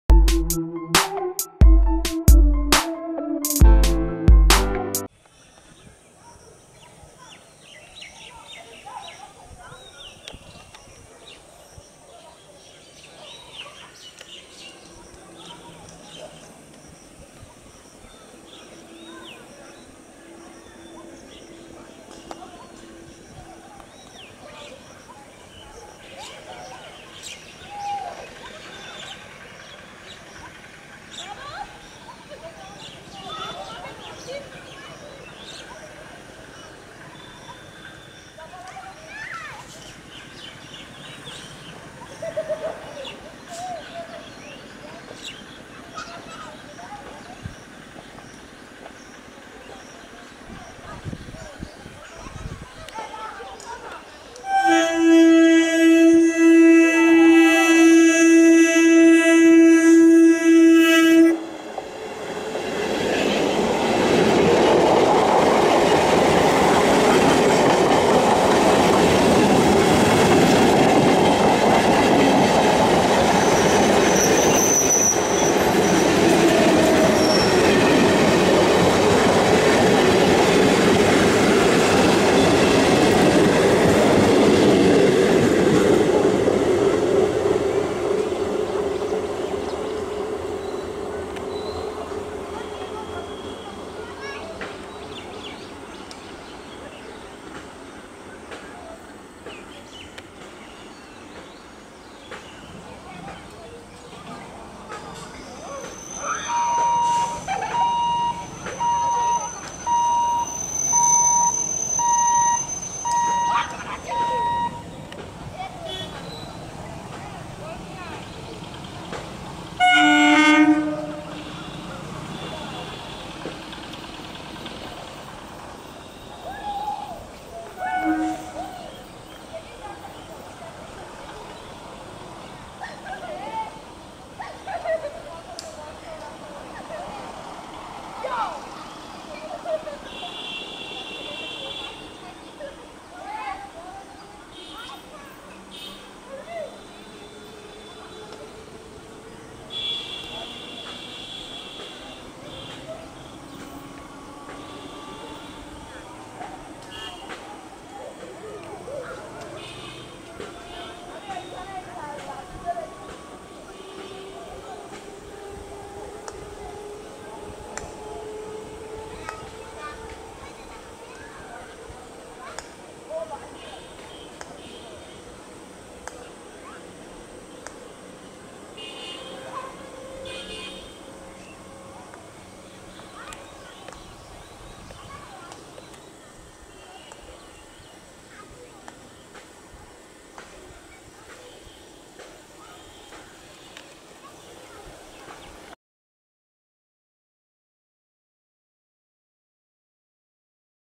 Train in lockdown || Indian Railways Staff only special train in Bandel-Katwa section
Device : Sony HX 400V